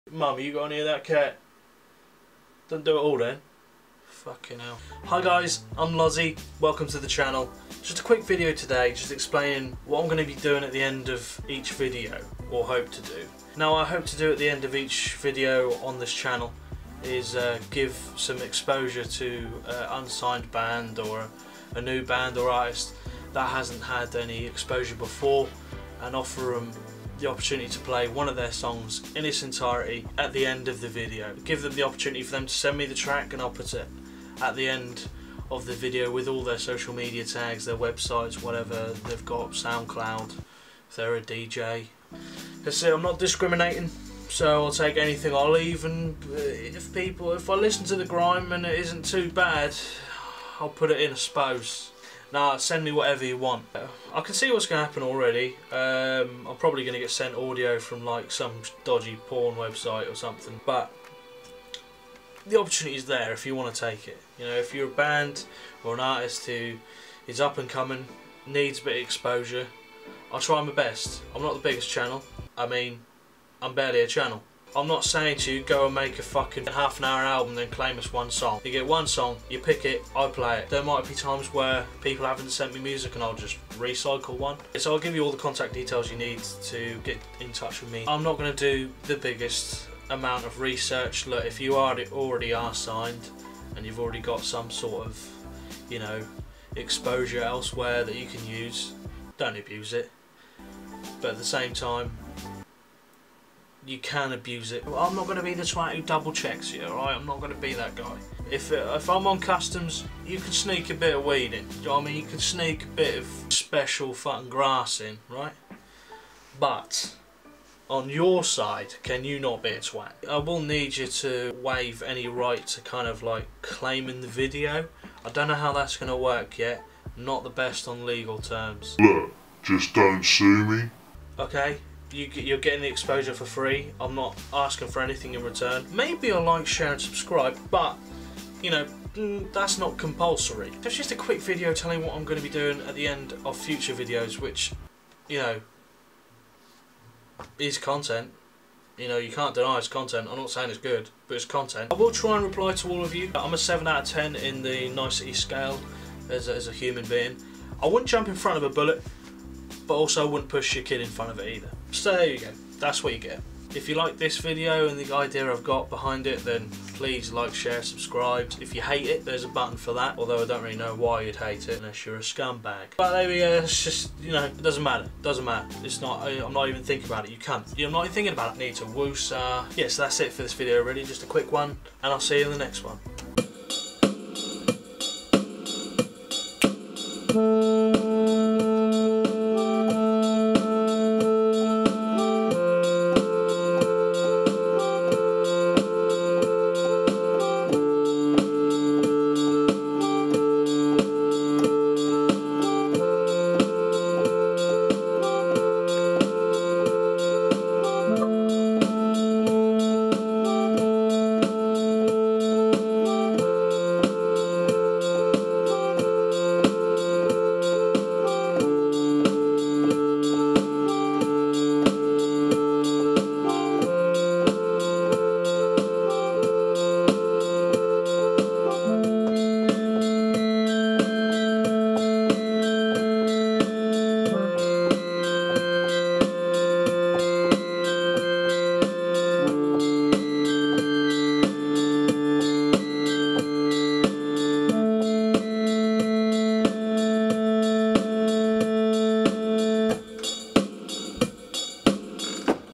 Get Your Music In My Videos
So I have had an idea. At the end of my videos I'm offering any artists who aren't signed and who are independent the opportunity to have one of their songs placed at the end of my videos. In this video I explain how you can take up this opportunity.